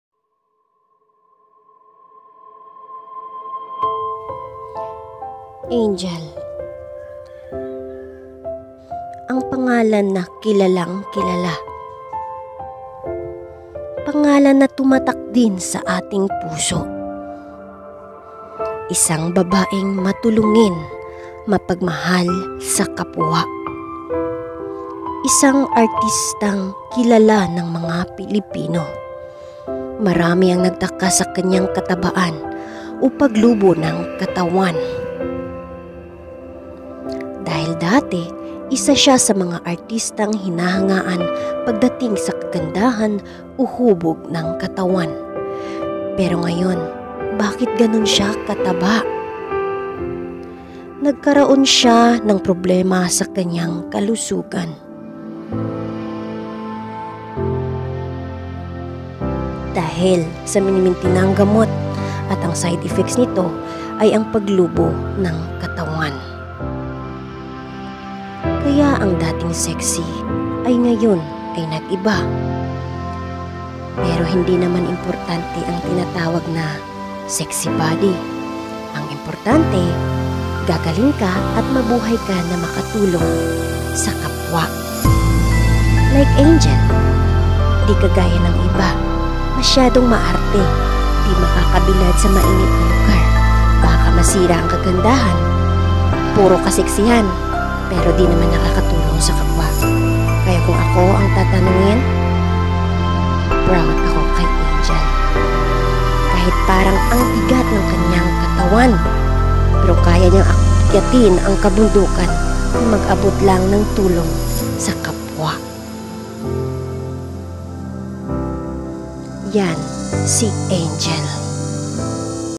Ang katutuhan sa Pagka Lubo o pag taba ni Angel
Mga taong masyadong mapang husga